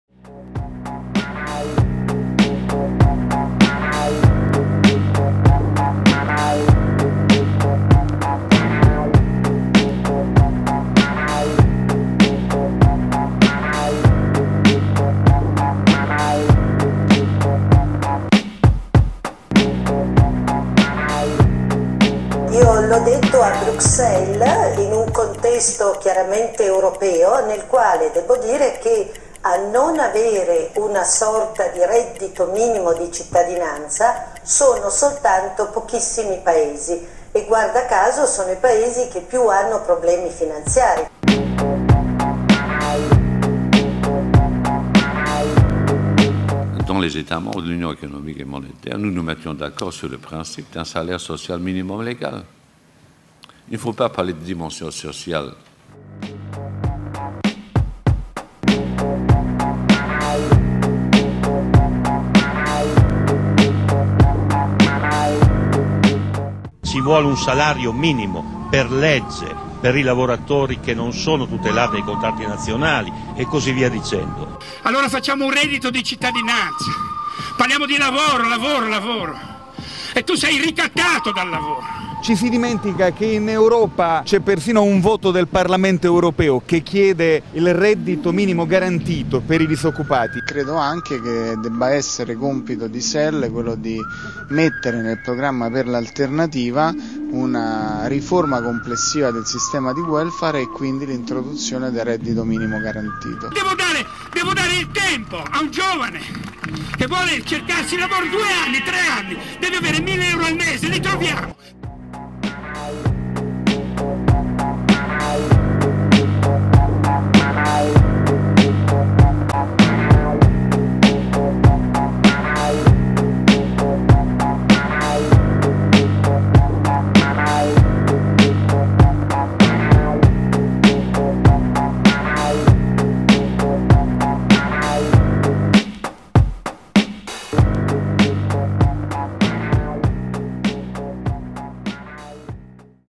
anzituttoredditopertutti
Non abbiamo più tempo di aspettare. Vogliamo un reddito di base e incondizionato che ci dia la possibilità di rifiutare il ricatto della precarietà. Siamo millioni di precari, disoccupati, lavoratori autonomi, a progetto...una classe in divenire che non ha niente da perdere. LA NOSTRA PRIORITÀ NON È UN GOVERNO, MA UN REDDITO PER TUTTI